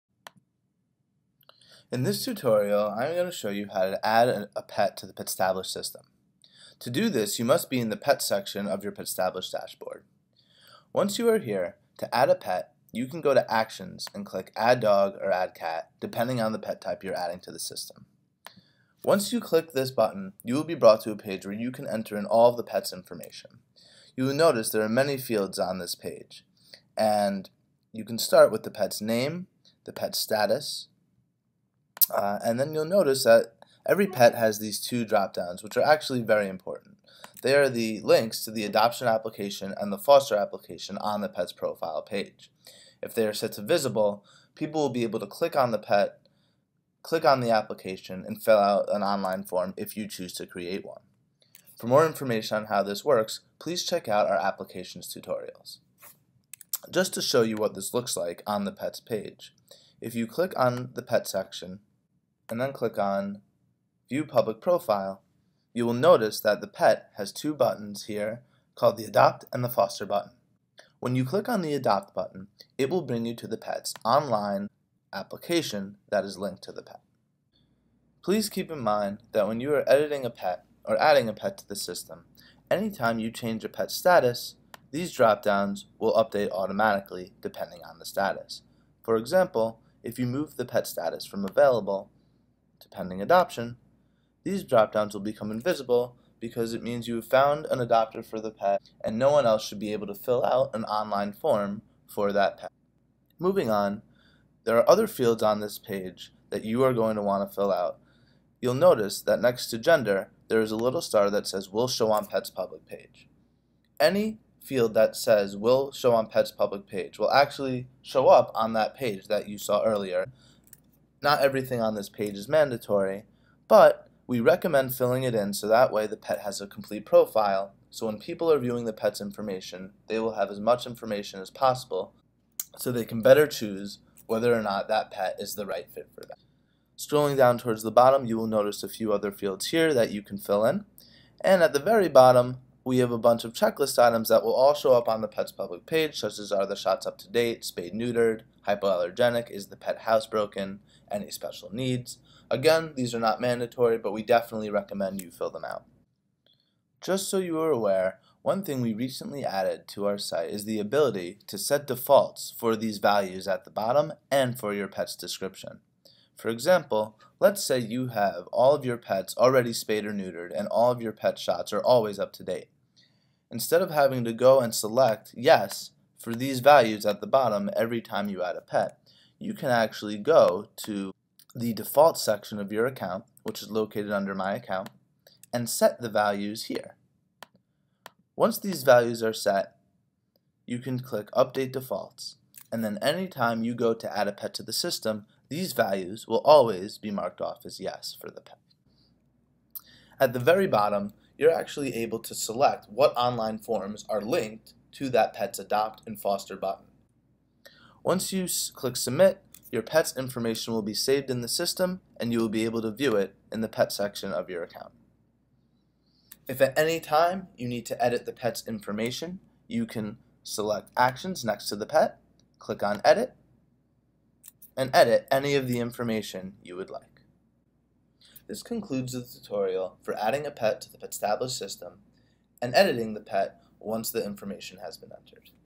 Adding and Editinga Pet
This tutorial shows you how to add a Pet to the Petstablished system and edit the pet once it is added. FAQ which are answered in this tutorial:

1. How do I add a pet to the system?
2. How do I edit a pet after it is added?
3. What does it mean when I set the Adoption and Foster Application drop downs to invisible?
4. How do I assign one of my online applications to a specific pet's profile page.